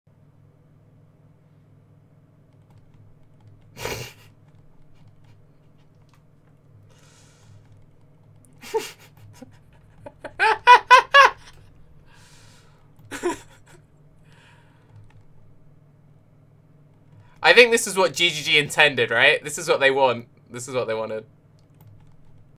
Beachhead Rota Viable? WTB Particle Sliders.
So I took the Magma Orb Sire of Shards Berserker into beachhead rotations and for some reason I was met by hostility and confusion by many of my fellow group members. Please end GFX bigotry by introducing particle sliders GGG.